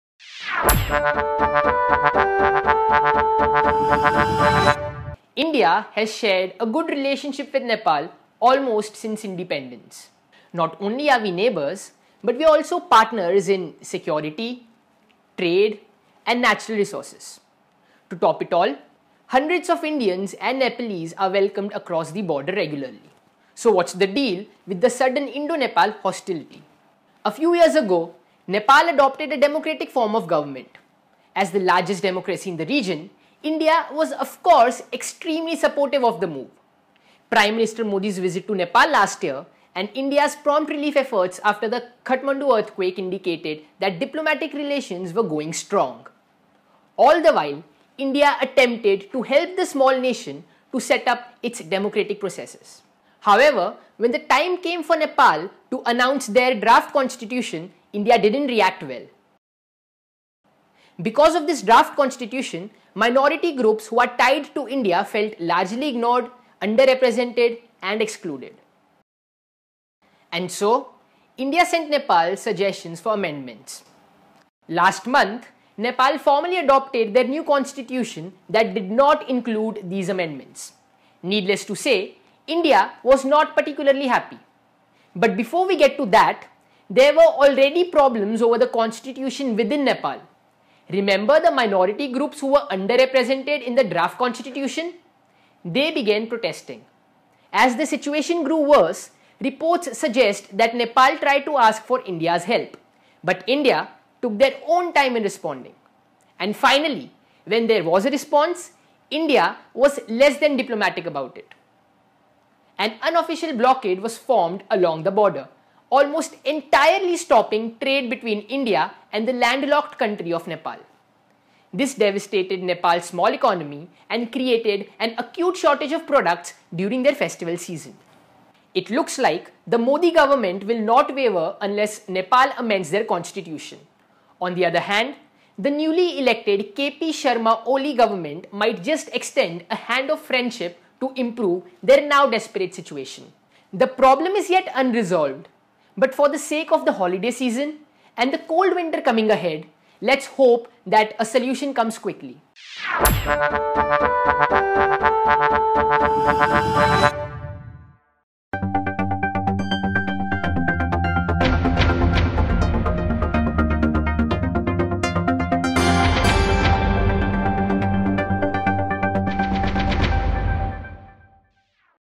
Nepal's Constitution & India's "Trade Blockade"
2015 has been a year of strained relations between India and Nepal. As Nepal adopted their new Constitution, violent protests of the Madhesi people broke out in the Terai region to object to their recognition and representation under the document.

Afraid of spillover violence in the neighbouring state of Bihar, which is currently in the process of electing it's new Legislative Assembly, India formed a "blockade" along the border, preventing essential items like fuel & groceries from entering this small Himalayan nation-state.

Meanwhile, the Narendra Modi Government hopes that Nepal's Prime Minister, KP Sharma Oli, will amend the Constitution to be more inclusive of their entire population.